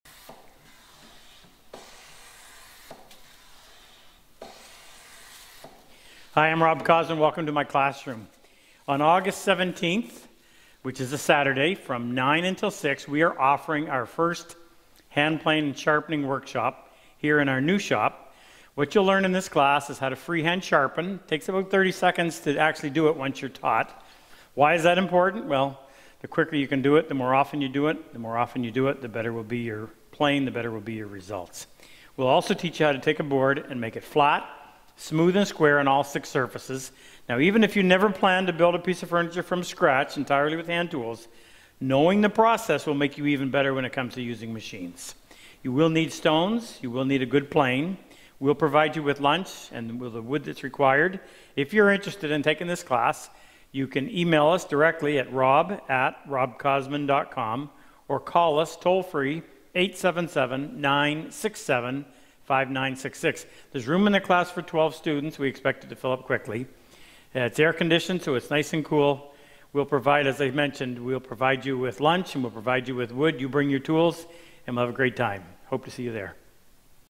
Sharpening and Hand Planing Class August 17, 2019
Rob and crew are offering their first sharpening and hand planing class, August 17, from 9 am to 6 pm.  Room for 12, each student gets their own bench.  You need to bring stones, a coarse and a fine, a good plane, jack, fore or jointer.  We supply the wood and a great lunch.  Location is 275 River Valley Dr., Grand Bay, NB.  About one hour from the Calais, Me border crossing.